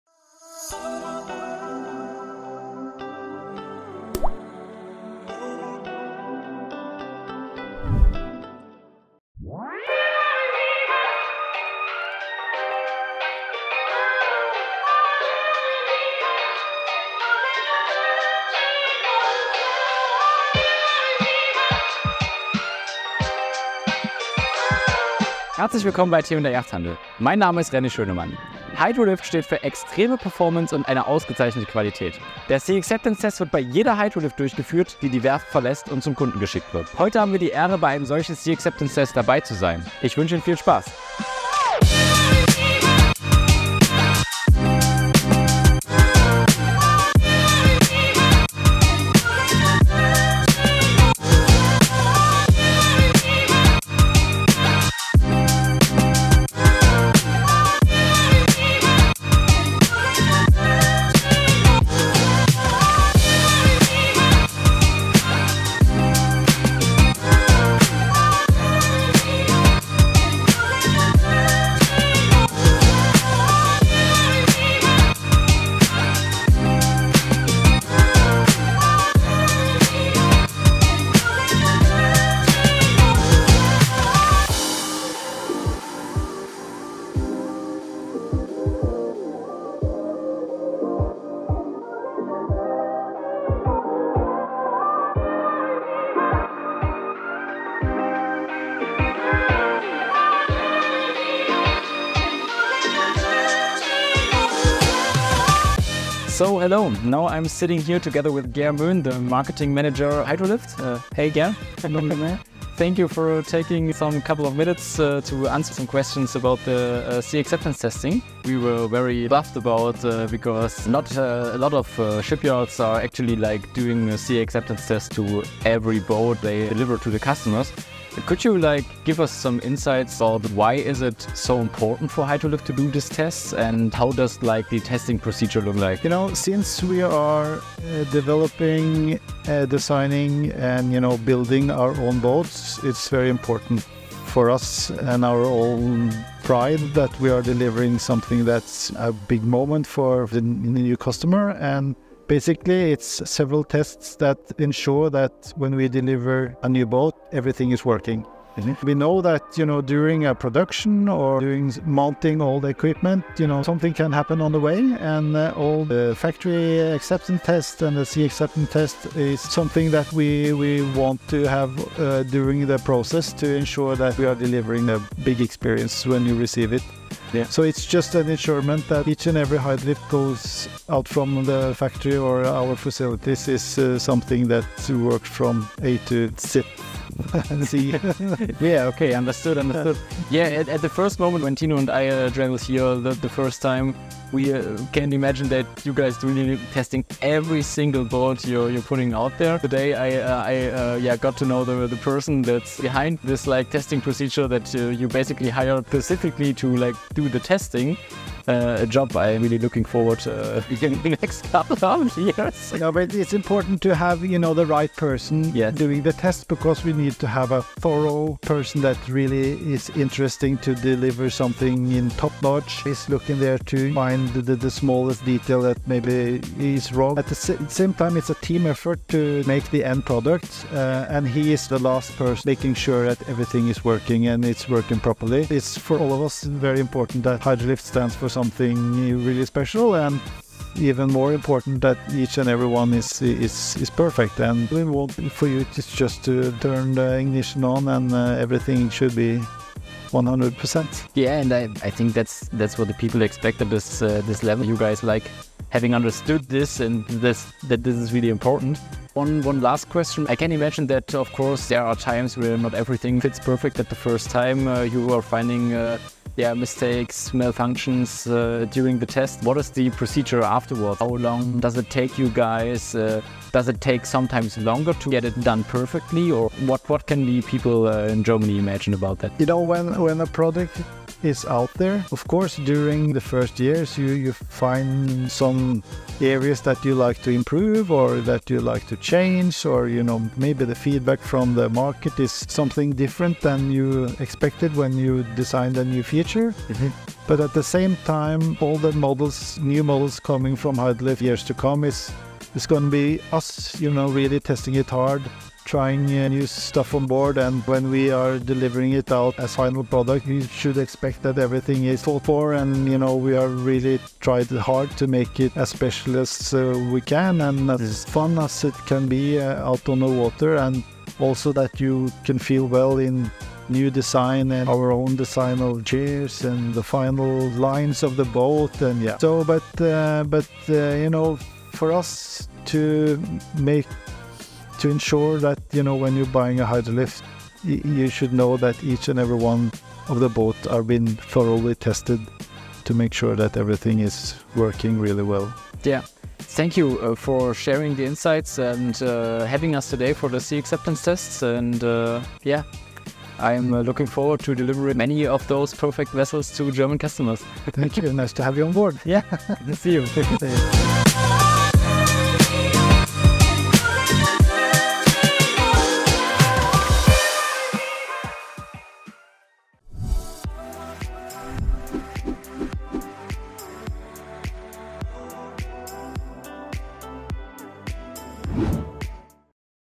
WAS? JEDES BOOT WIRD GETESTET? Bei hydrolift ist das STANDARD! - 2/3
Bei hydrolift wird jedes Boot im Wasser getestet, bevor es zum Kunden geht. Das ist Standard, um die meisterhafte Verarbeitung sicherzustellen. Wir haben einen Test begleitet und waren von der hohen Sorgfalt überrascht. Der Ehrgeiz des Testers, auch jeden kleinsten Fehler zu finden und ausbessern zu lassen hat uns zum staunen gebracht.
hydrolift steht für meisterhafte Verarbeitung und der Sea Acceptance Test ist ein elementarer Baustein dafür.

Mehr Infos gibt es das erste Mal exklusiv auf der Boot&Fun INWATER in Werder!

T&R Yachthandel GmbH
An der Fine 8
06842 Dessau-Roßlau

Steuernummer: 114/108/07698
Stendal HRB 22517